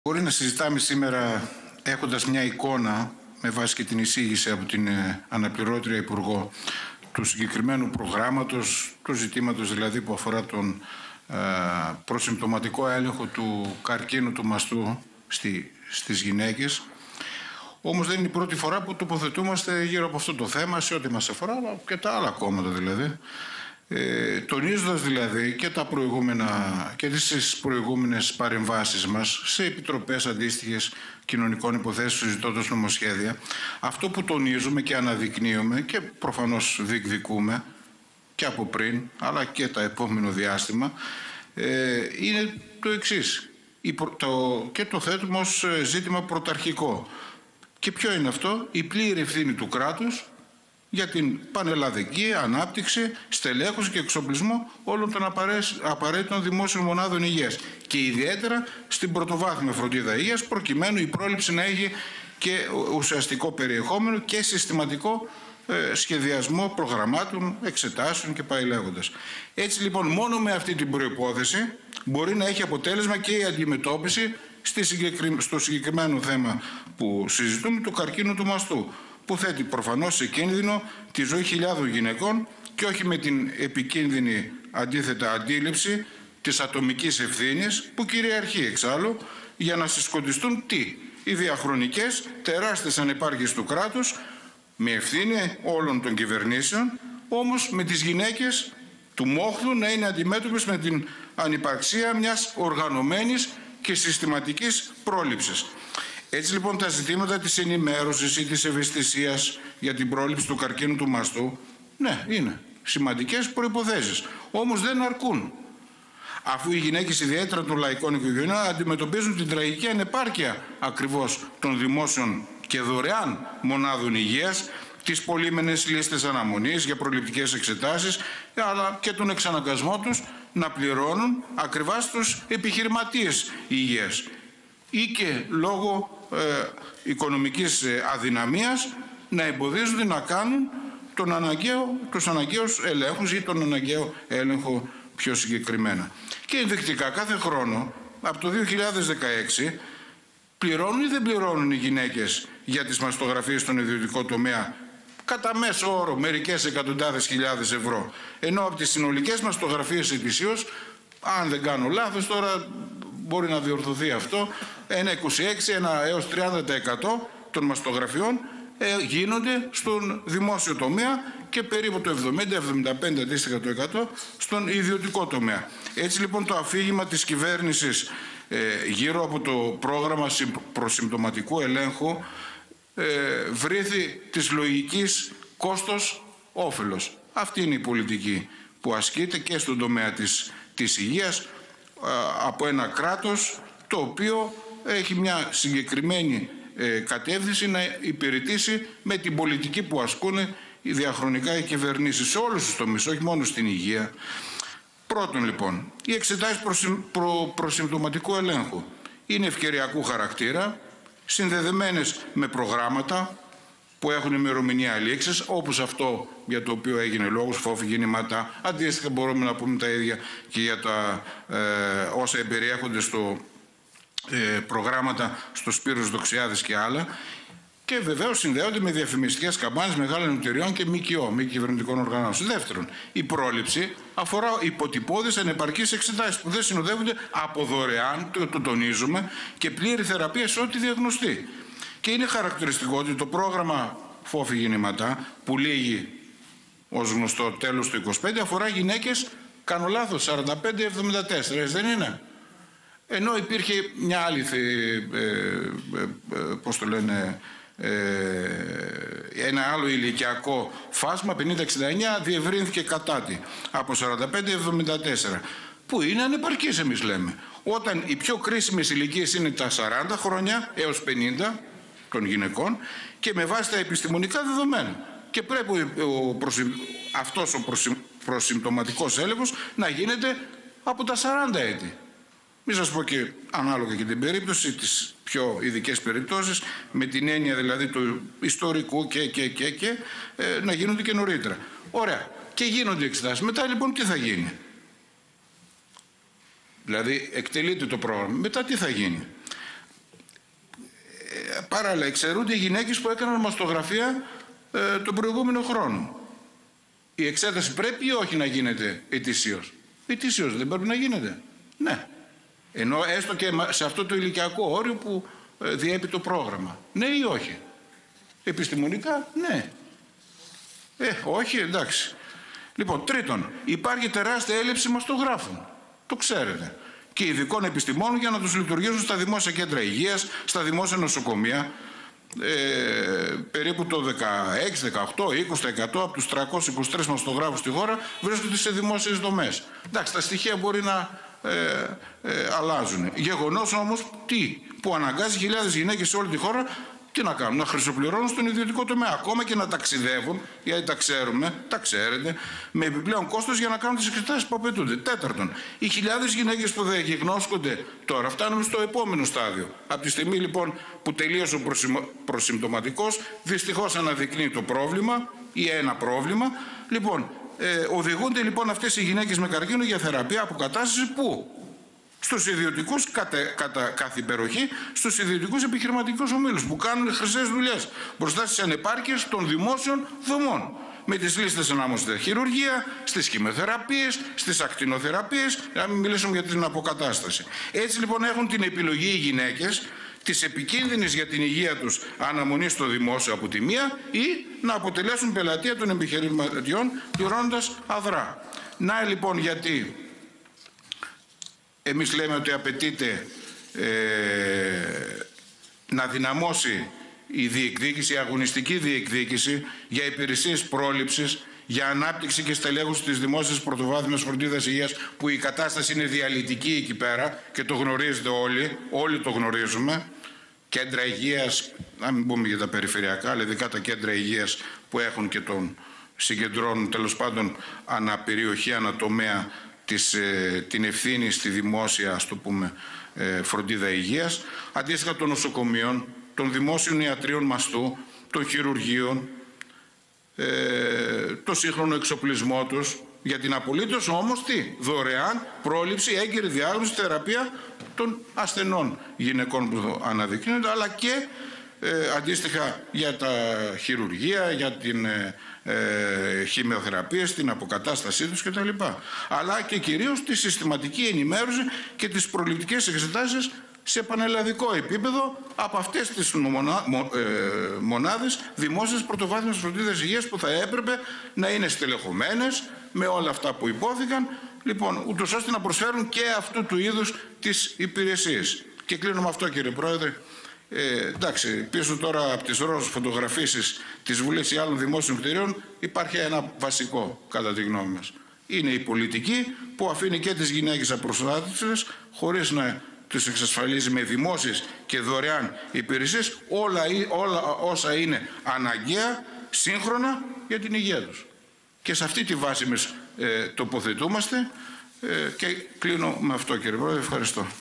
Γ. Λαμπρούλης: Η πολιτική για τη πρόληψη του καρκίνου του μαστού βρίθει της λογικής κόστος-όφελος
Η πανελλαδική ανάπτυξη, στελέχωση και εξοπλισμός δημόσιων μονάδων Υγείας, ειδικά των πρωτοβάθμιων, ώστε να γίνεται έλεγχος με ουσιαστικό περιεχόμενο και συστηματικό σχεδιασμό προγραμμάτων και εξετάσεων, είναι σημαντική προϋπόθεση για την αποτελεσματική αντιμετώπιση του καρκίνου του μαστού, σημείωσε ο *Γιώργος Λαμπρούλης,* βουλευτής του ΚΚΕ, μιλώντας στο πλαίσιο σχετικής συνεδρίασης της Επιτροπής Κοινωνικών Υποθέσεων της Βουλής.

Αντίθετα, όπως είπε, η επικίνδυνη αντίληψη της ατομικής ευθύνης στοχεύει «να συσκοτιστούν οι διαχρονικές τεράστιες ανεπάρκειες του κράτους, με ευθύνη όλων των μέχρι τώρα κυβερνήσεων».

Εξήγησε, πιο συγκεκριμένα, ότι η ενημέρωση και η ευαισθησία για την πρόληψη του καρκίνου του μαστού είναι εξίσου σημαντικές προϋποθέσεις, αλλά δεν αρκούν. Όπως περιέγραψε, χιλιάδες γυναίκες λαϊκών οικογενειών καλούνται να επιλέξουν για τις προληπτικές εξετάσεις τους ανάμεσα στις πολύμηνες λίστες αναμονής λόγω των τεράστιων ανεπαρκειών στις δημόσιες και δωρεάν μονάδες Υγείας (π.χ.: μαστογράφοι και αντίστοιχο εξειδικευμένο προσωπικό που τους λειτουργεί), στο να ακριβοπληρώσουν επιχειρηματίες Υγείας και στο να μην κάνουν τους αναγκαίους ελέγχους λόγω οικονομικής δυσκολίας.

«Το αφήγημα της κυβέρνησης γύρω από το πρόγραμμα προσυμπτωματικού ελέγχου βρίθει της λογικής κόστος-όφελος», τόνισε ο Γ. Λαμπρούλης, λέγοντας ότι οι εξετάσεις του είναι ευκαιριακού χαρακτήρα και συνδέονται με προγράμματα με ημερομηνία λήξης και με διαφημιστικές καμπάνιες μεγάλων εταιρειών και ΜΚΟ, ενώ δεν συνοδεύονται από δωρεάν και πλήρη θεραπεία σε ό,τι διαγνωστεί, με αποτέλεσμα να οδηγούνται «καθ' υπεροχή» σε ιδιωτικούς επιχειρηματικούς ομίλους «που κάνουν χρυσές δουλειές» πάνω στις ίδιες ανεπάρκειες και λίστες αναμονής αλλά, αυτή τη φορά, για χειρουργεία και χημειοθεραπείες.

Ειδική αναφορά έκανε και στο γεγονός του φάσματος των υπό εξέταση ηλικιών που καλύπτει το πρόγραμμα «Φώφη Γεννηματά», δηλαδή γυναίκες στα 45-74, επισημαίνοντας ότι επιστημονικά ο προσυμπτωματικός έλεγχος πρέπει να γίνεται ήδη από τα 40 και κάθε χρόνο, σε αντίθεση με την πρόβλεψη του προγράμματος να εξαιρούνται γυναίκες που έκαναν μαστογραφία τον προηγούμενο χρόνο!

Ο Γ. Λαμπρούλης διατράνωσε την απαίτηση για αγωνιστική διεκδίκηση για υπηρεσίες πρόληψης,  για ανάπτυξη και στελέχωση δημόσιας πρωτοβάθμιας φροντίδας Υγείας, νοσοκομείων, δημόσιων ιατρείων μαστού, χειρουργείων, με σύγχρονο εξοπλισμό και για την απολύτως δωρεάν πρόληψη-έγκαιρη διάγνωση-θεραπεία, κόντρα στην «πολιτική που αφήνει και τις γυναίκες απροστάτευτες χωρίς να τις εξασφαλίζει με δημόσιες και δωρεάν υπηρεσίες όλα όσα είναι αναγκαία, σύγχρονα για την υγεία τους».